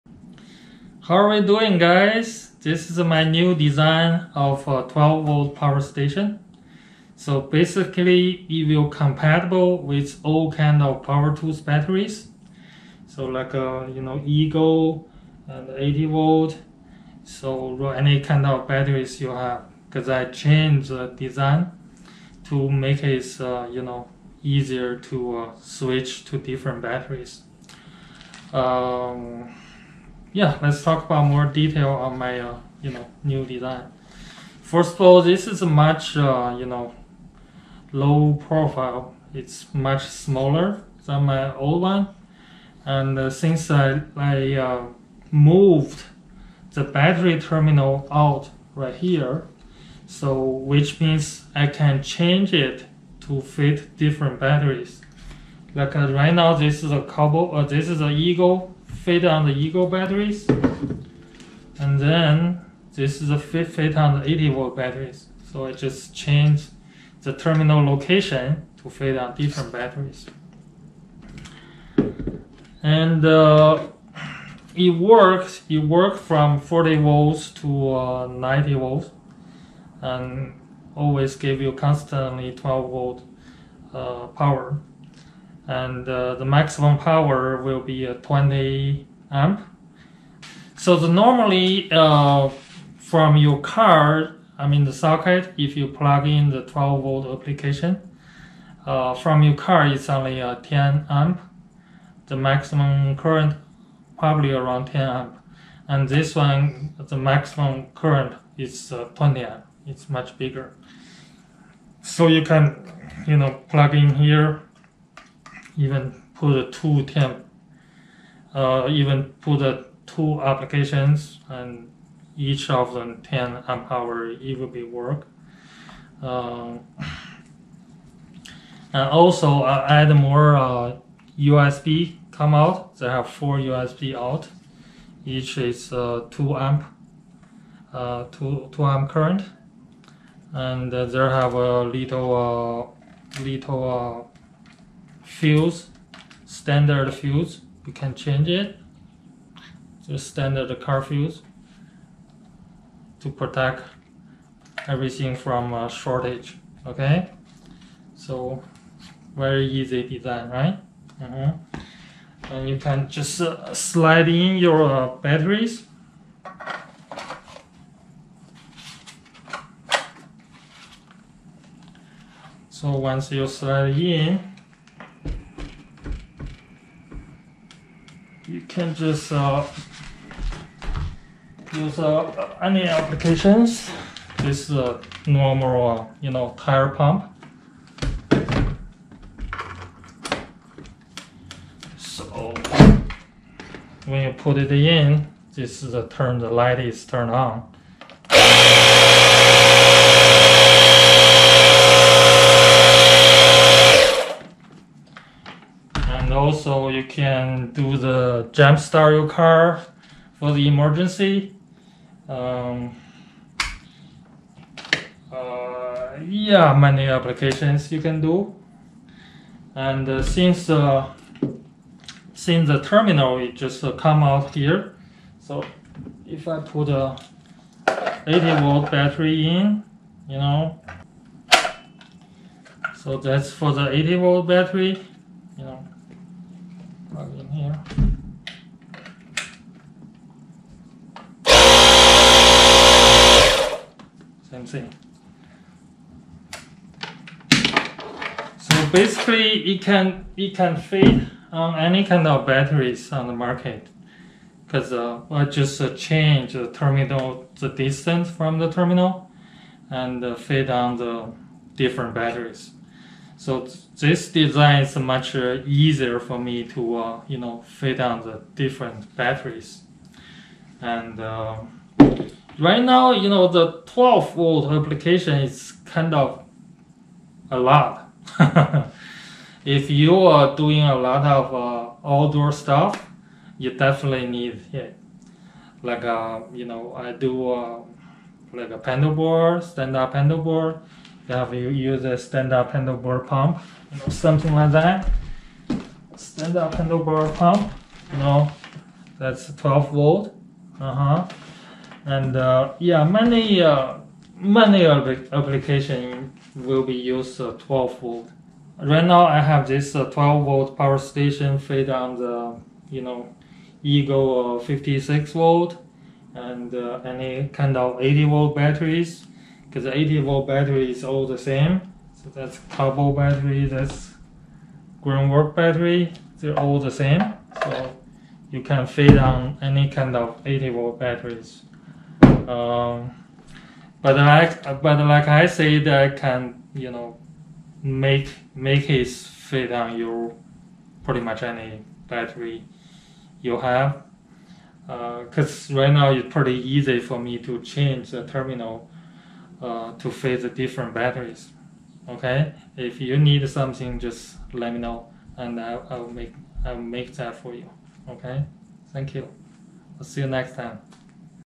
EGO 56V Battery to 12V Converter Power station
New low profile design, will link more different brands of batteries to 12v + 5v power station.
Built tough with double 12V DC power outlet(15A) & 4 USB ports(2A)
Convenient comparable to any 80v battery, and EGO 56v battery (more on the way)
Power up all 12V appliances and 5V USB
Must have for travel: emergency charge car battery and jump start